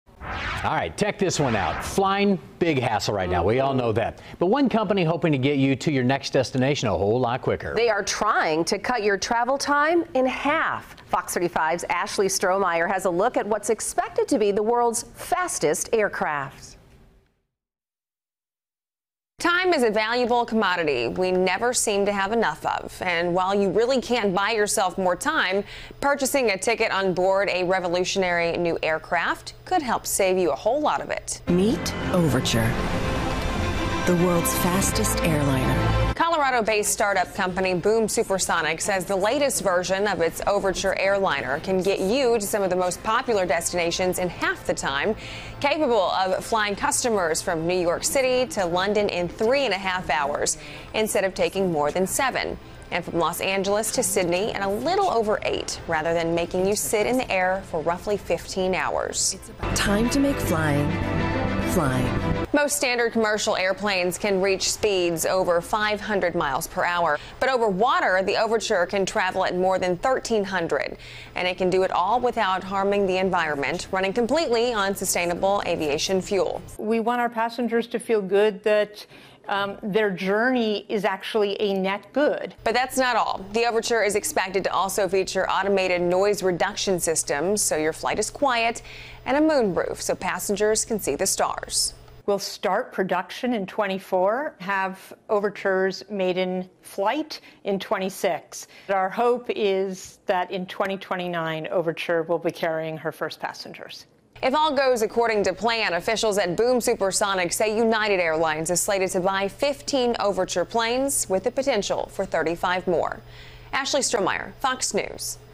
Supersonic flight: Aircraft aims to fly from New York to London in three hours
Boom Supersonic is setting out to redefine commercial travel by bringing supersonic speed to flights.